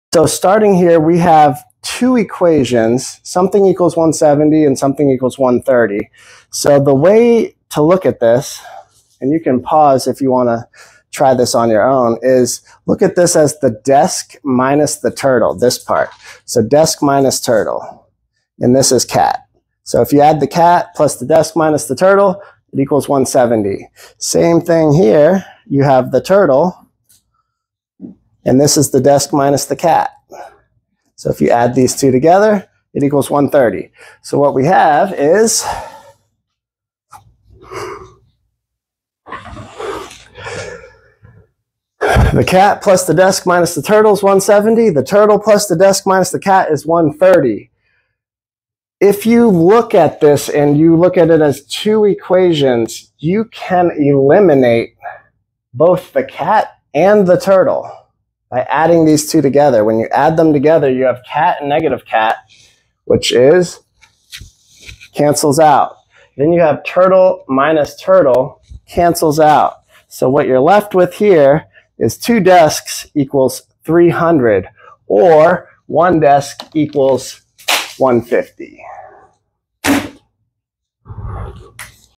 How tall is the desk?
people have told me this can not be answered because there are two equations and 3 unknowns.  But you can!